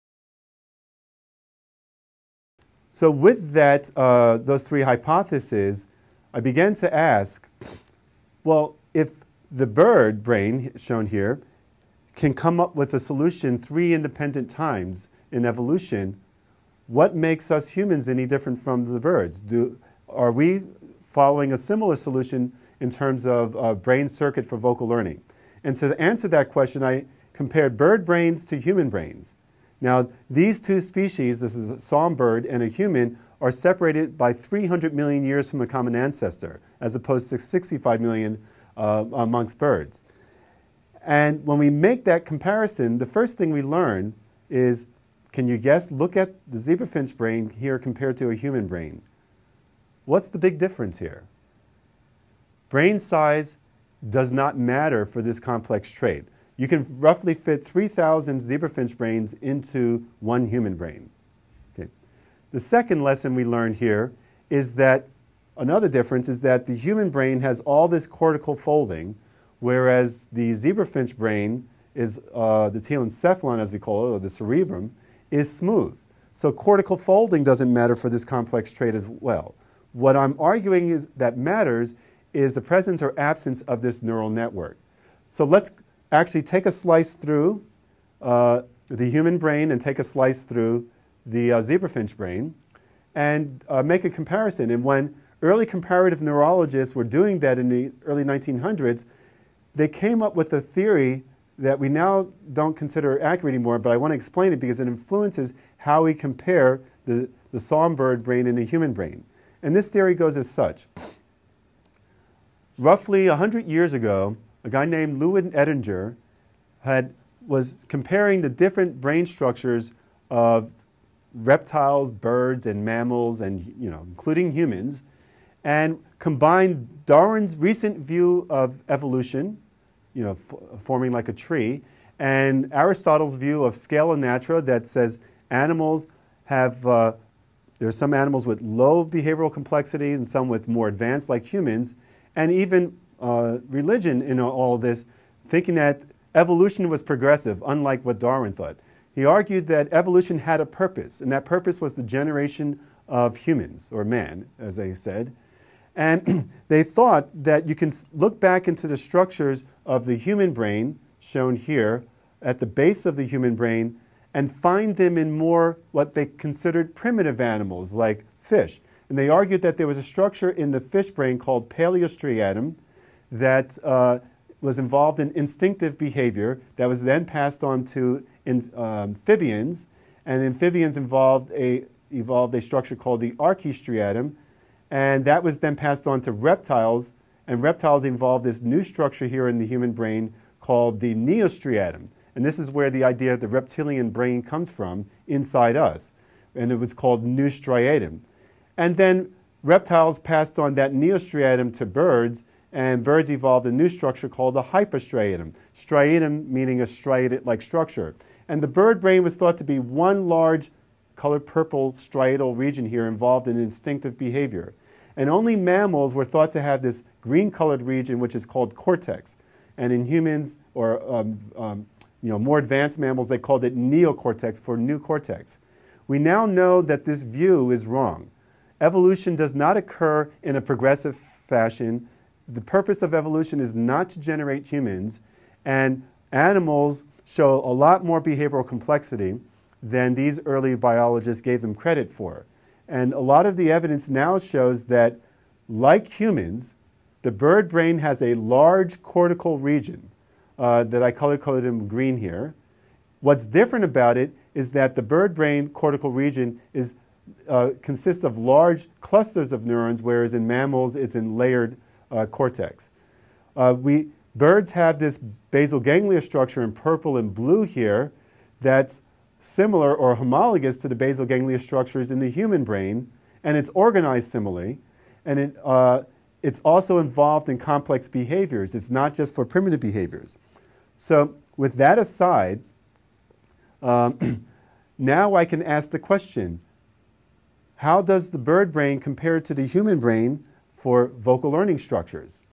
Bird and mammal brain homologies - Erich Jarvis (Duke/HHMI)
Brain homologies across species of vocal learners have been viewed differently over time.